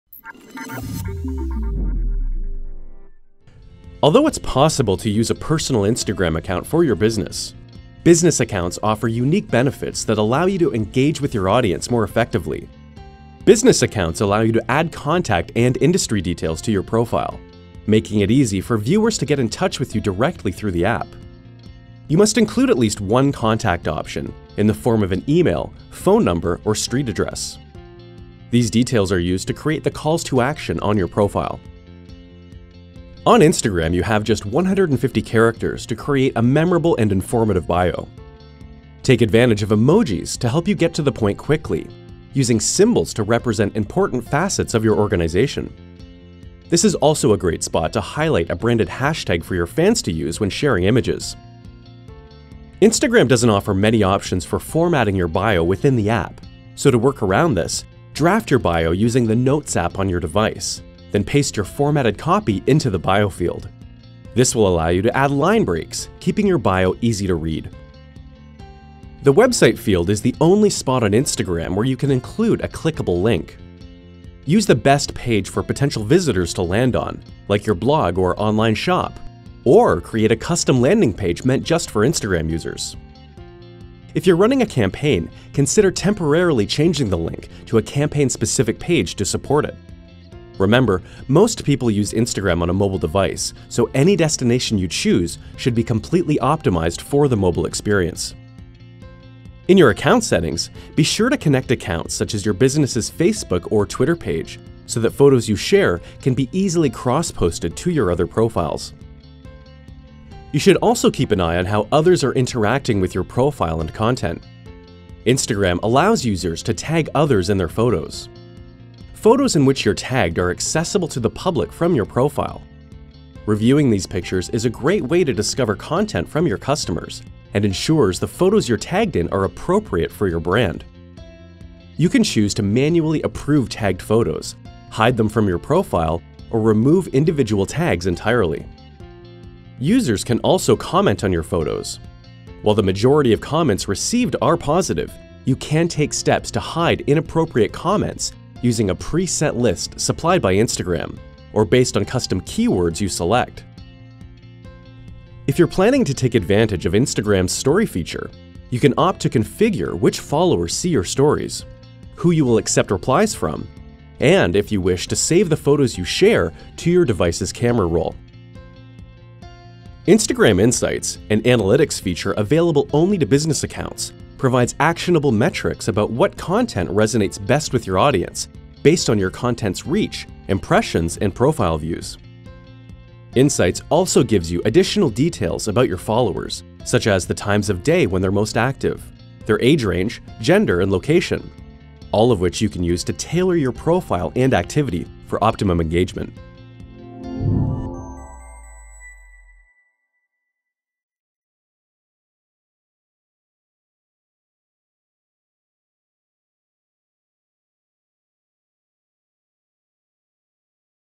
How to Optimize Your Instagram Business Profile
Note: This is a sample video from our comprehensive Social Marketing Certification course at Hootsuite Academy. Get educated and certified today!

Although it‘s possible to use a personal Instagram account for your business, business accounts offer unique benefits that allow you to engage with your audience more effectively.

In this video, learn valuable tips for optimizing your Instagram profile, including managing tagging, comments, and optimizing your Instagram bio.